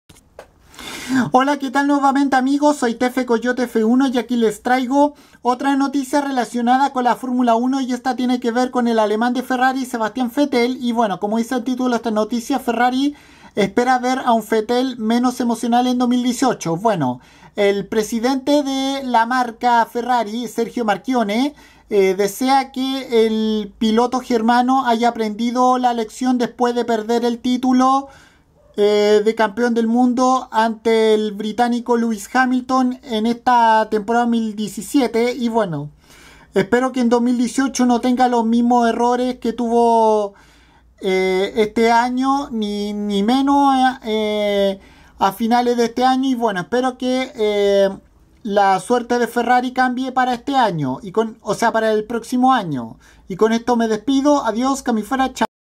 Ferrari espera ver a un Vettel menos emocional para 2018
El presidente de la marca Ferrari Sergio Marchionne desea que el alemán Sebastian Vettel haya aprendido la lección después de perder contra Hamilton el título este 2017 y que pueda peliár por el en 2018